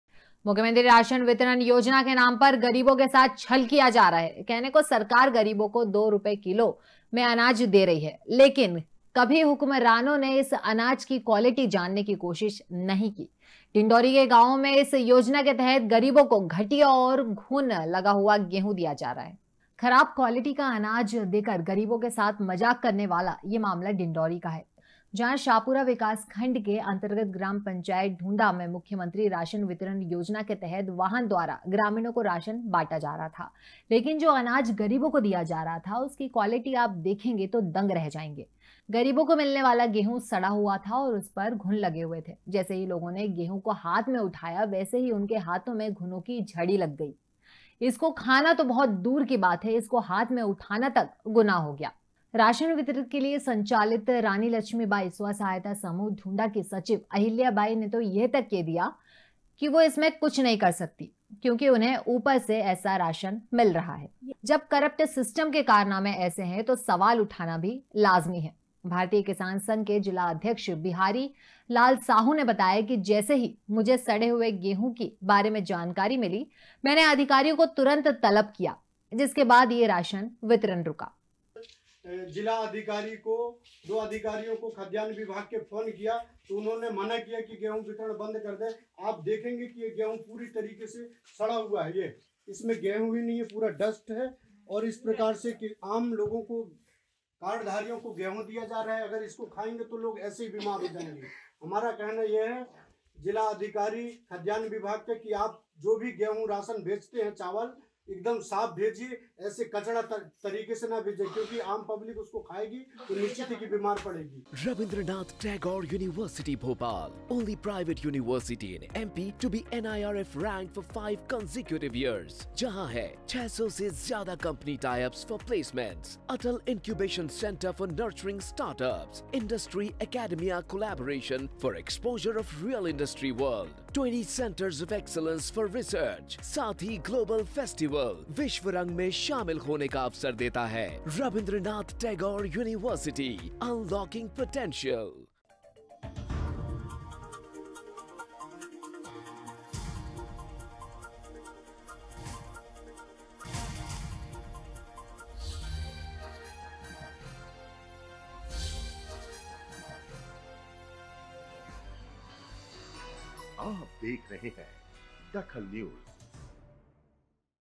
DAKHAL NEWS डिंडोरी में खराब अनाज वितरित कर रही है सरकार
डिंडोरी में खराब अनाज वितरित कर रही है सरकार
राशन वितरण योजना में मिल रहा है सड़ा हुआ गेहूं